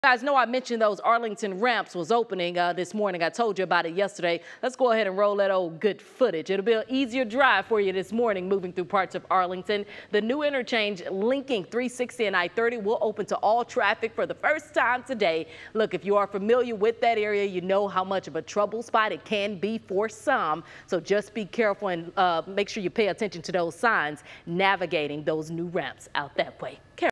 Arlington, TX: New SH 360 interchange to I-30 open Thursday
TxDOT says the new northbound State Highway 360 interchange will connect to east and westbound Interstate 30.